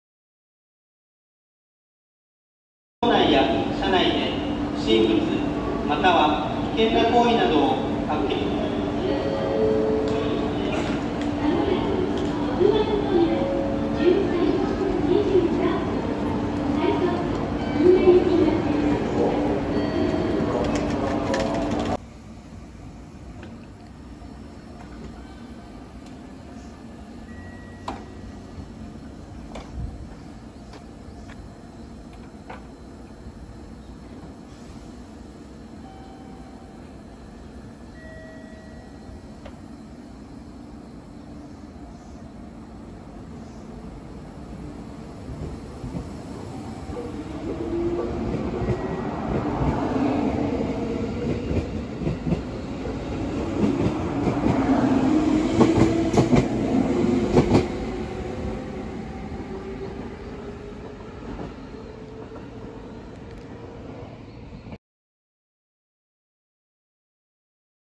７８３系ＣＭ３ ９０６５Ｍ臨時特急かもめ６５号佐賀行 鹿児島本線博多駅幕回し＆長崎本線新鳥栖駅発車
２０１９年１２月３０日撮影

年末年始期間の臨時特急かもめ号です。

この日で、７８３系５両編成充当の、２０１９年〜２０２０年年末年始の運転が終わり、次回は、２０２０年ＧＷ期間の運転となります。